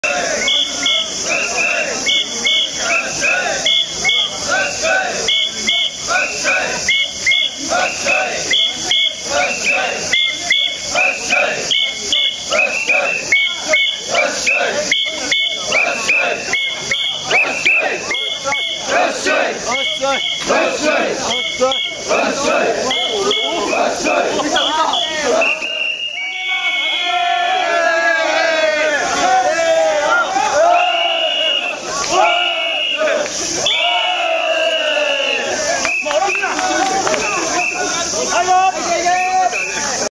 2012.07.29 宇佐夏越祭り お上り（還御）行列 その４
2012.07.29　宇佐夏越祭り　お上り（還御）の時代行列。　三之神輿も参道で練り。